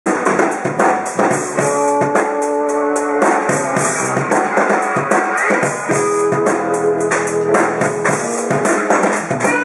Belleville Blues Band - mars 2012 - 02
Au Vieux Saumur, 10 rue de Belleville 75020 Paris, le 29 mars 2012 à 21h